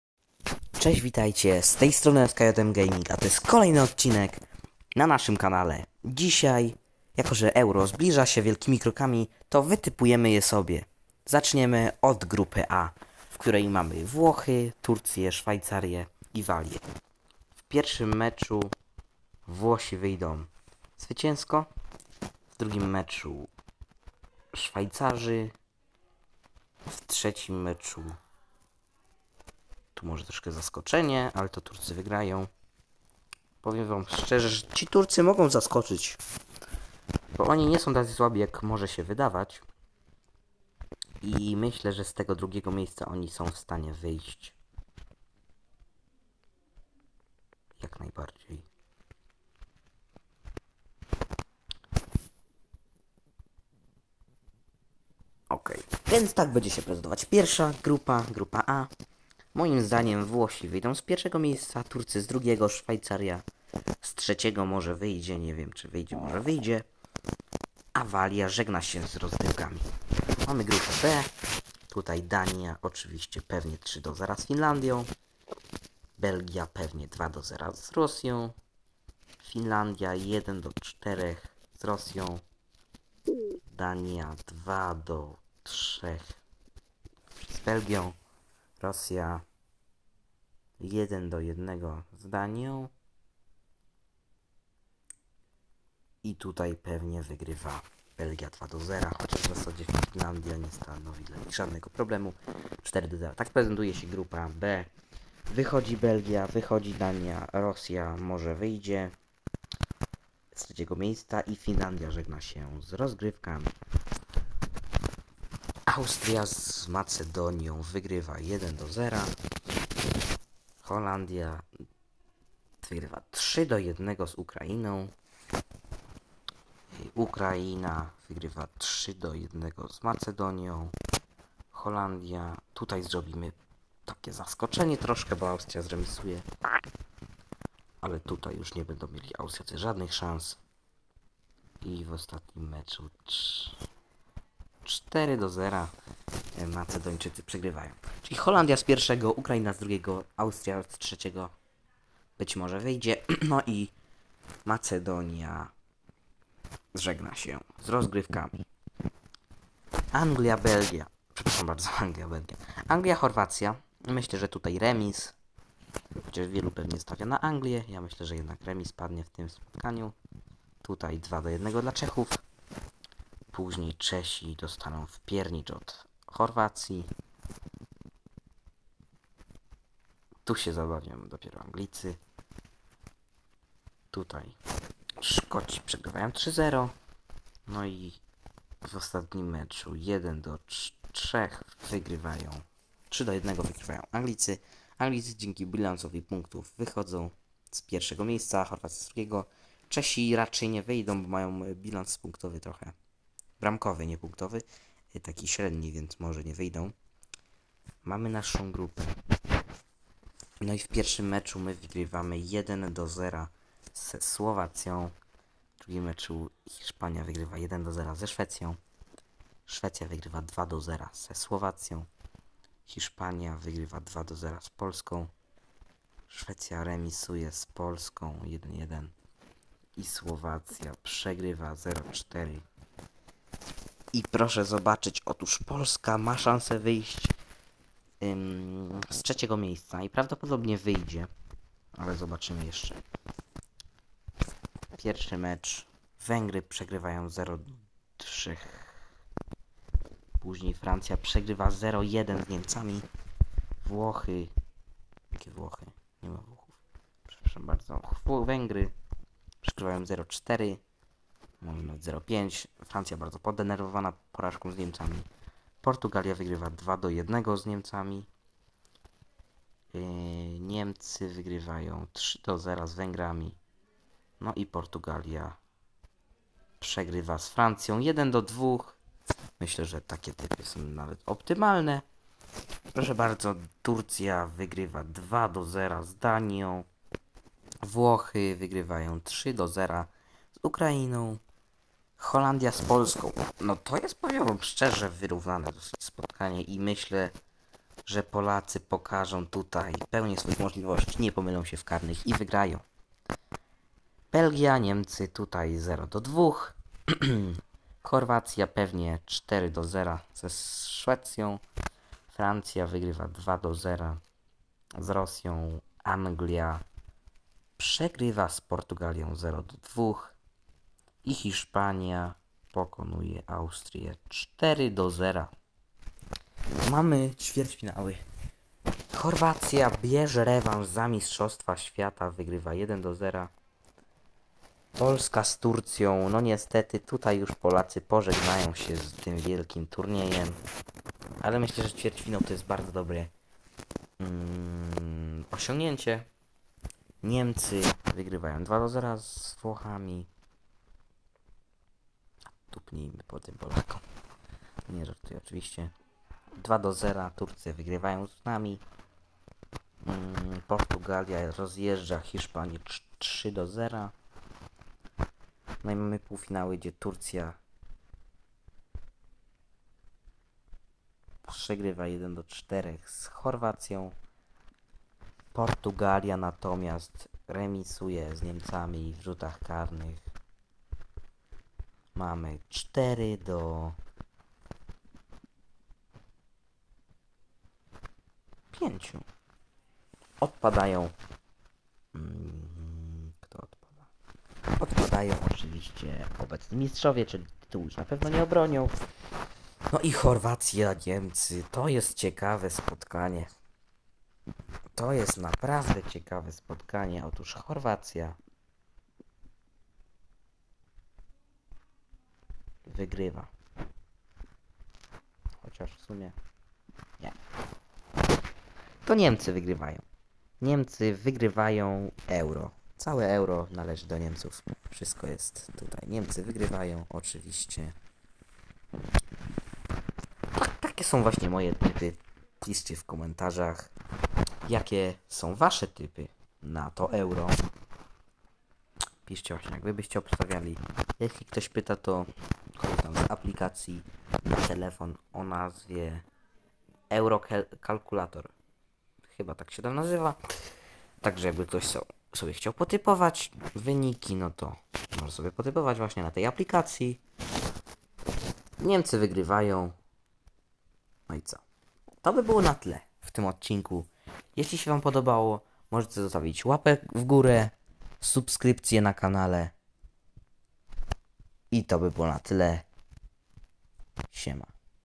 Moje typy na Euro 2020
W tym filmie wytypowałem wyniki euro 2020.Jeśli odcinek się wam podobał możecie zostawić subskrypcję i łapkę w górę. To by było na tyle trzymajcie się na razie cześć.